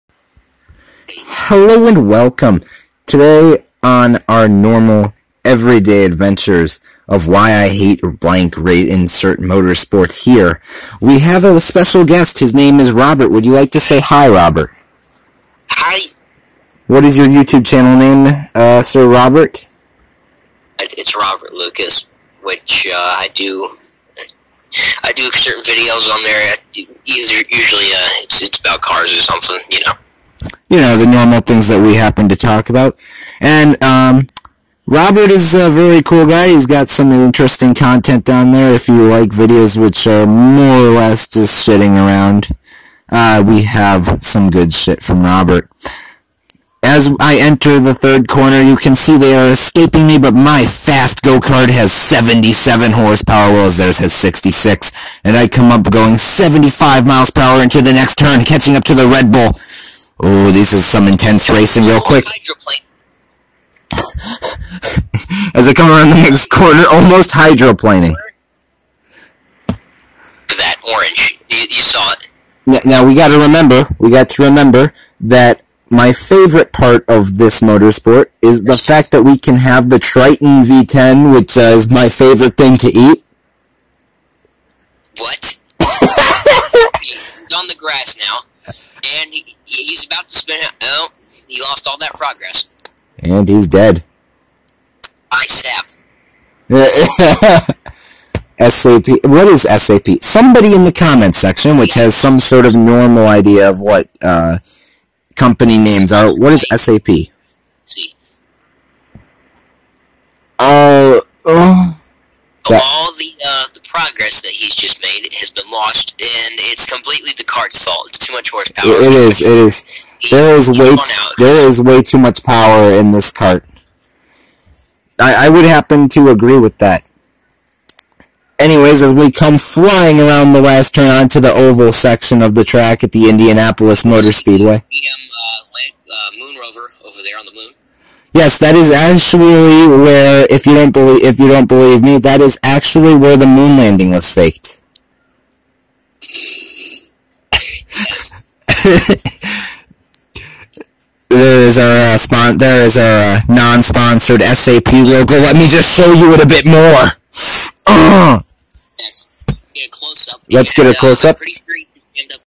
Why I Hate KARTING (Rotax Max at Indy)
Special guest Robert Lucas